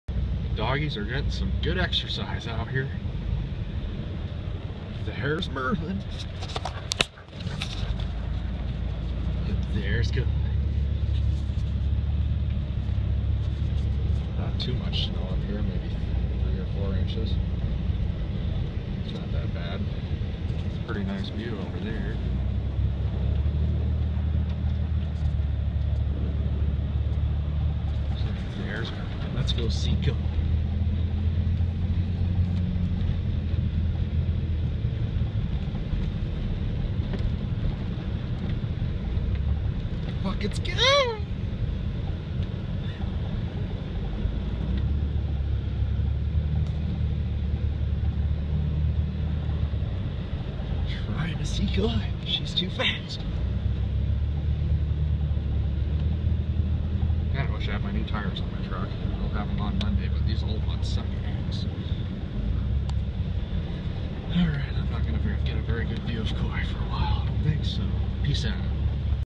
Snow wheeling Tacoma and dogs.
Playing around on the mountain yesterday. Got stuck and had a good old time!